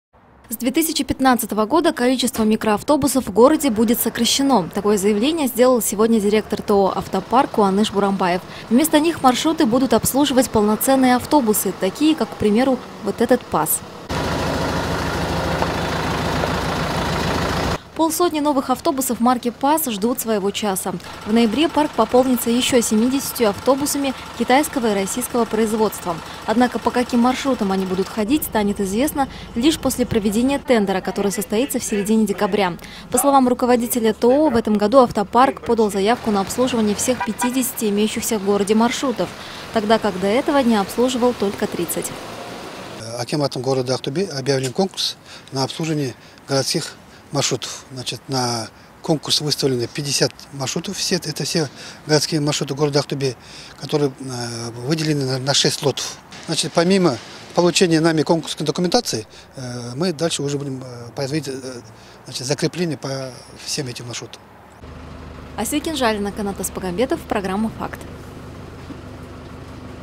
На дорогах города появятся новые автобусы
До конца года ТОО «Автопарк» обновит свой парк 120 автобусами российского и китайского производства.
С 2015 г. количество микроавтобусов в городе будет сокращено, такое заявление сделал директор ТОО «Автопарк» Куаныш Бурамбаев. Вместо них маршруты станут обслуживать автобусы - первые 50 «ПАЗиков» средней вместимости появились в Актобе уже сегодня.
В ноябре парк пополнится ещё 70 автобусами китайского и российского производства, однако по каким маршрутам они будут ездить, станет известно лишь после проведения тендера в середине декабря.
По словам руководителя ТОО, в этом году «Автопарк» подал заявку на обслуживание всех 50 имеющихся в городе маршрутов, тогда как до этого дня обслуживал только 30.